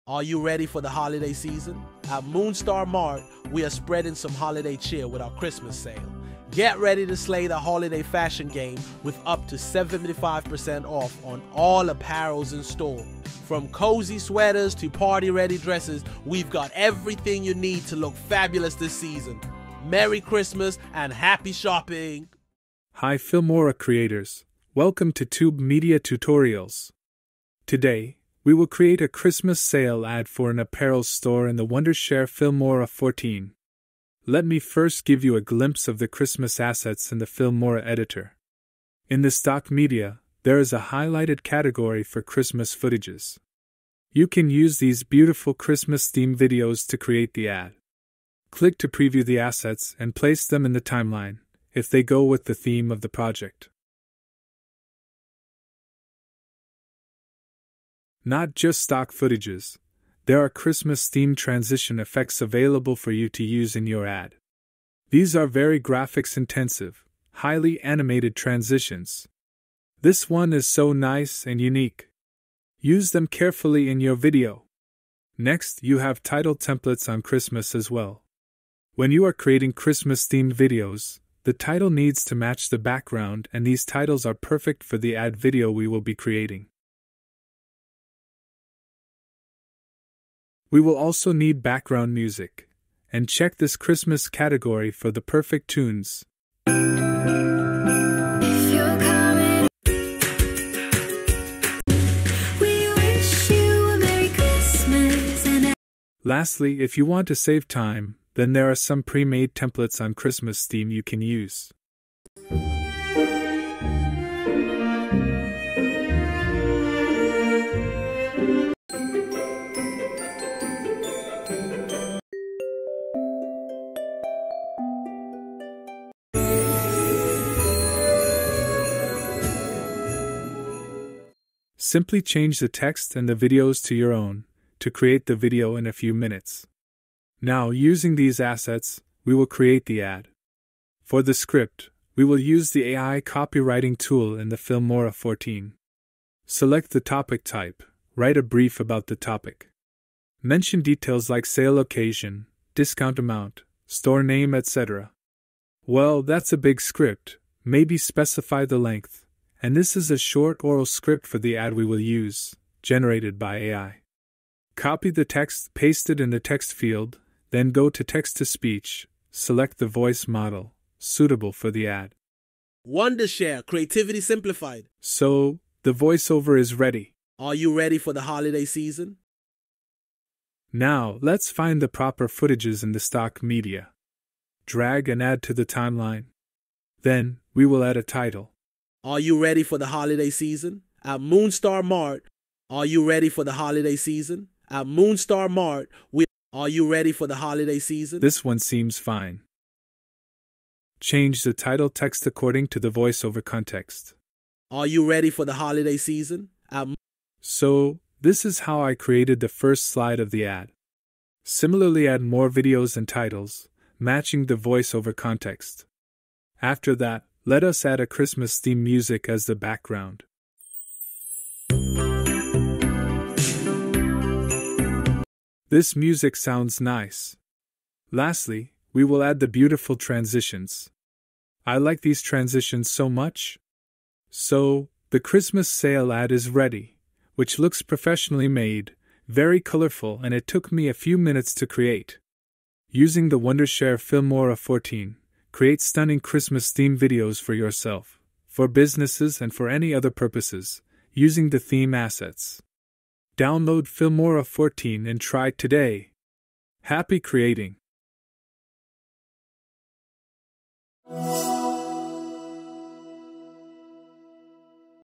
Create a Christmas Sale Advertisement in Filmora 14 | No Pro Editing Skill Needed!
Hi filmora creators, welcome to tube media tutorials! Today we will create an Ad for Christmas Sale, using the Filmora 14. The Christmas assets can be used to create stunning professional videos in minutes!